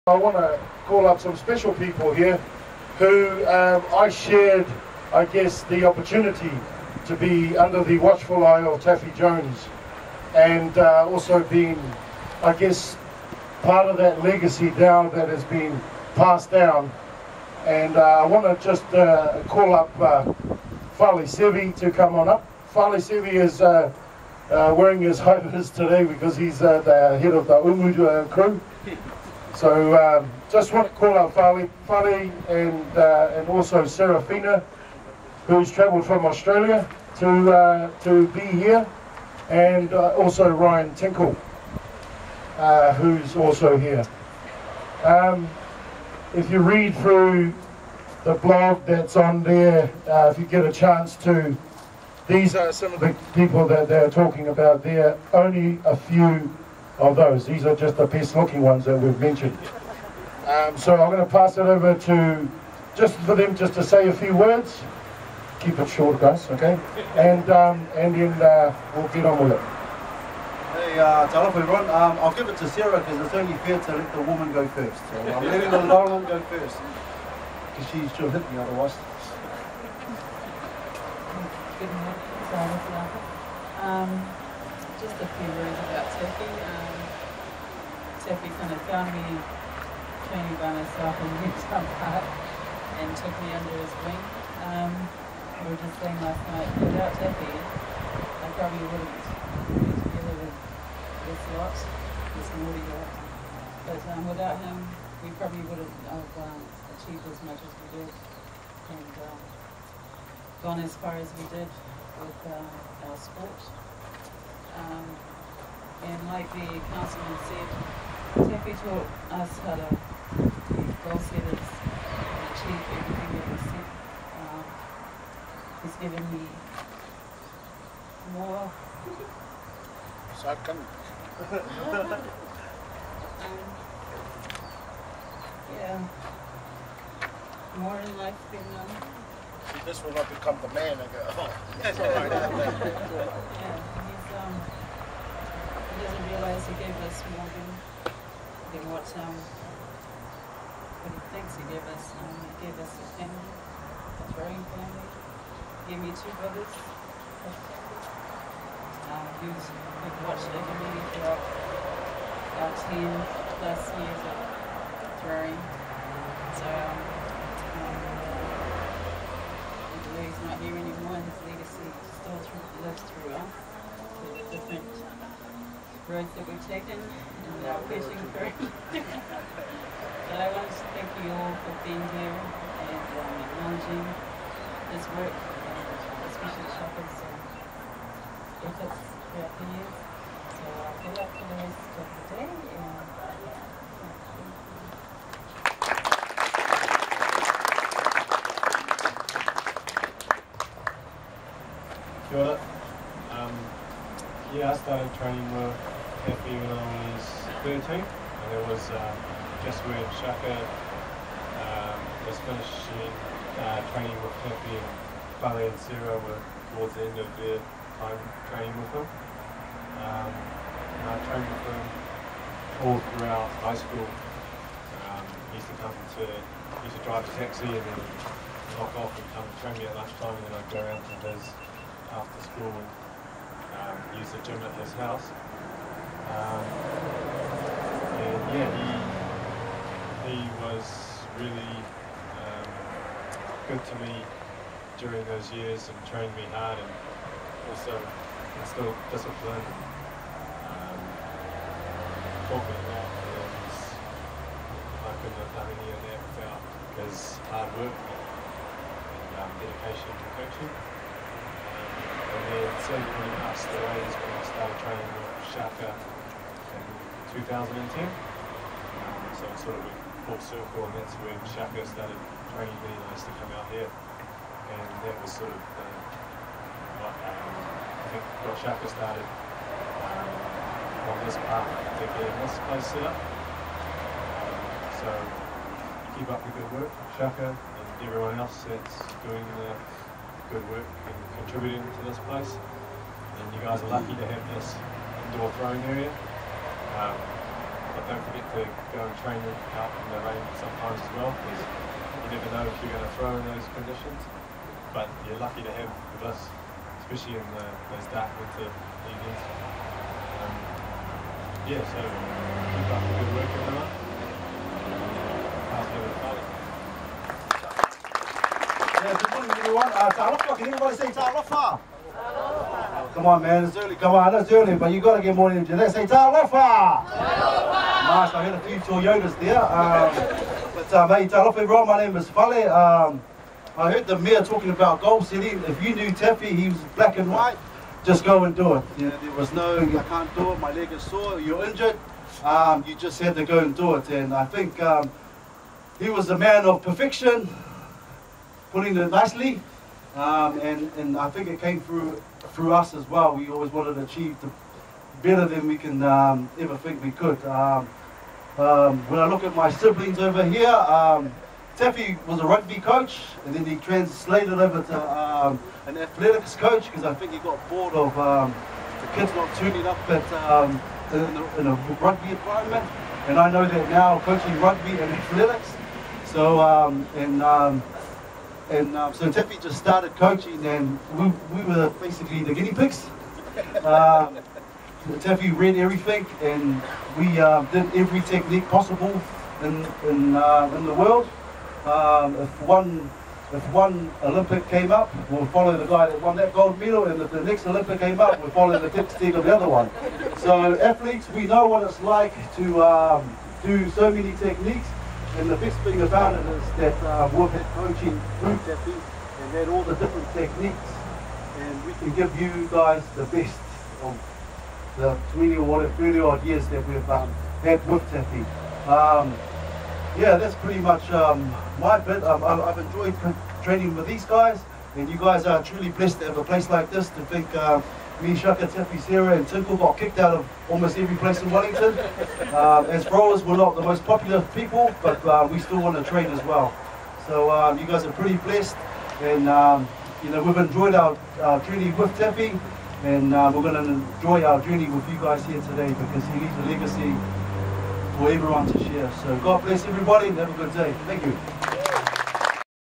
2018 SolaPower Throwing Academy Grand Opening of Taffy Jones Indoor Facility and Throwers Meeting Lower Hutt Sun 21 Jan Tributes to Taffy Jones